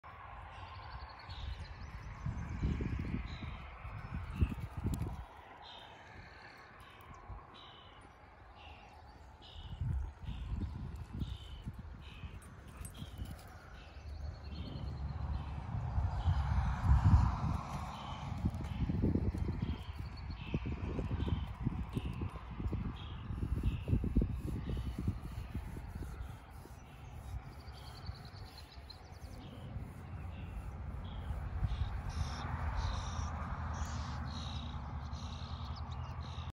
Fate Between Me And Lulu
Fate finds her way through the tightest places. This afternoon as she was "getting the sheep" she ran in the small space between me and Lulu. She is so agile and none of the animals, including the sheep, pay much attention to her. fullmoonfiberart.com